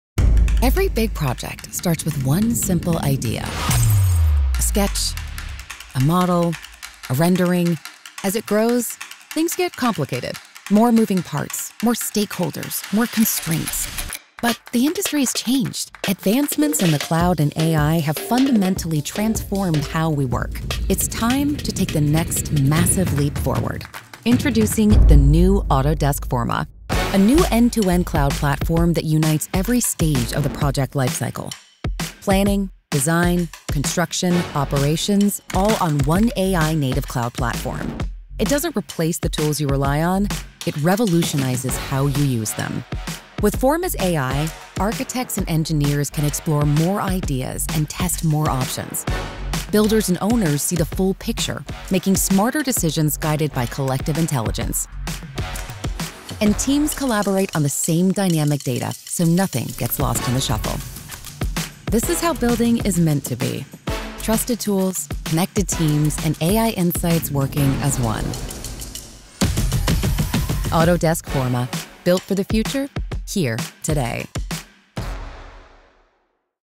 Autodesk Forma is Shaping the Future of AECO
We're bringing @AutodeskConstructionCloud proven capabilities into the @AutodeskForma industry cloud, creating the first end-to-end AECO industry cloud that delivers a complete lifecycle platform.

AutodeskForma is building a future that meets industry demand, where faster collaboration, smarter decisions, and better outcomes happen from start to finish.